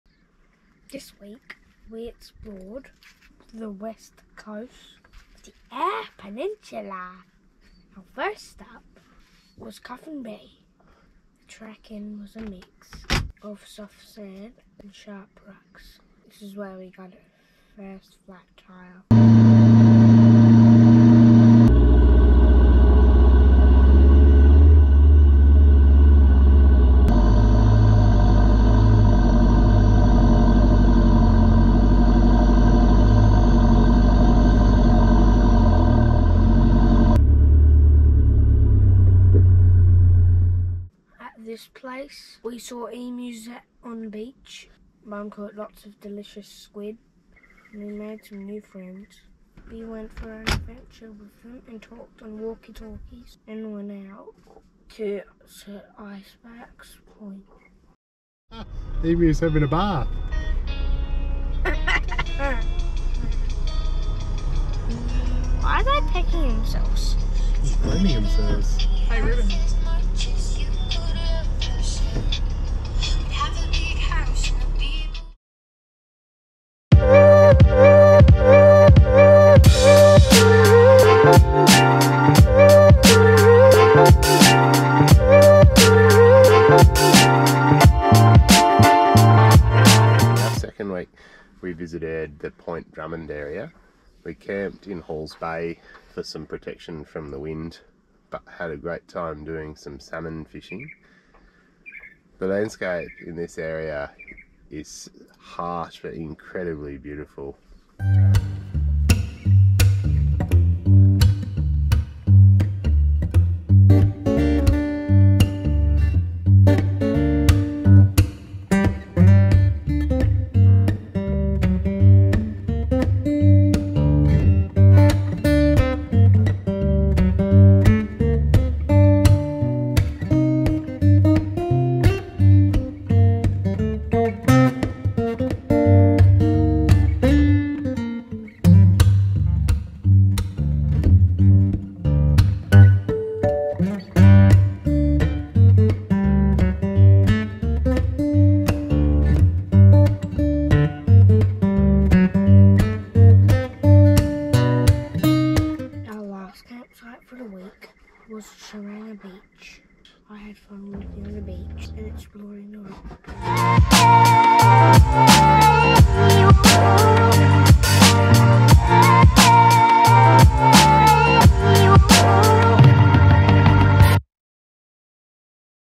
Week 2 - West coast of the Eyre Peninsula
This week we explored the west coast of the Eyre Peninsula. Beginning in the south we headed north and explored Coffin Bay National Park, Point Drummond & Hall Bay, finishing the week at Sheringa Beach. We caught and ate delicious seafood, were treated to dolphins on most of the days and met some great people who we enjoyed exploring and fishing with. The campsites in this area are amazing and the landscapes are rugged and beautiful. Bridget’s first attempt at squid fishing yielded us an enormous feed! We got our first flat tyre of the trip heading into Coffin Bay, but with some better quality tyres on the camper we are back off and exploring!